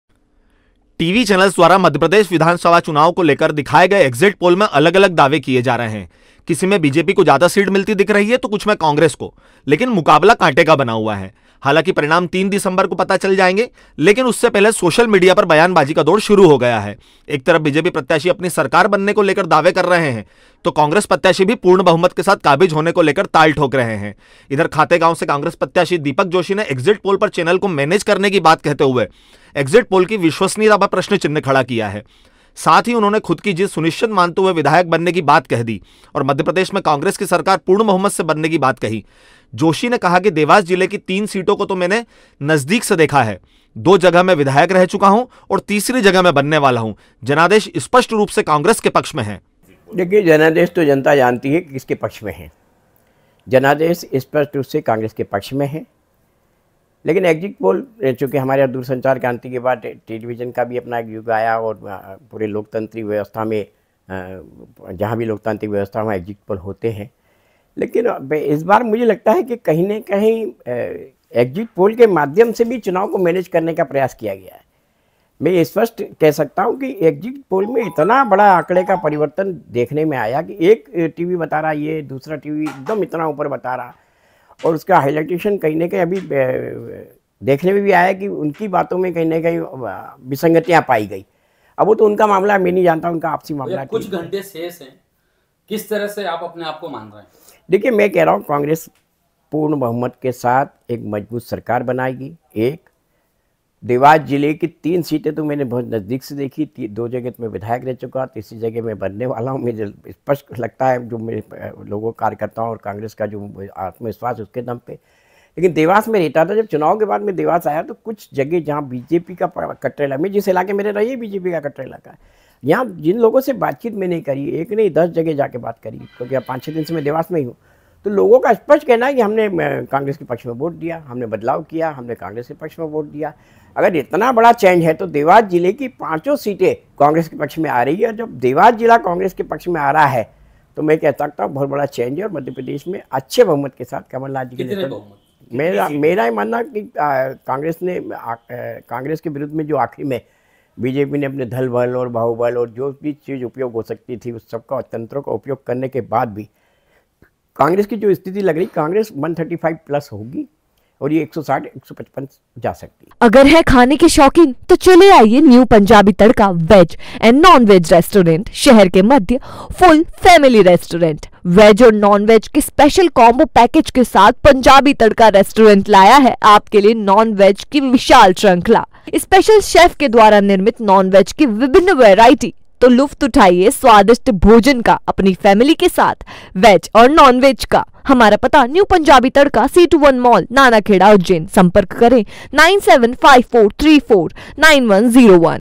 एग्जिट पोल के बाद सोशल मीडिया पर बयानबाजी शुरू | Dewas News
टीवी चैनल्स द्वारा मप्र विधानसभा चुनाव को लेकर दिखाए एक एग्जिट पोल में अलग-अलग दावे किए जा रहे हैं, किसी में बीजेपी को ज्यादा सीट मिलती दिख रही है तो कुछ में कांग्रेस को लेकिन मुकाबला कांटे का बना हुआ है।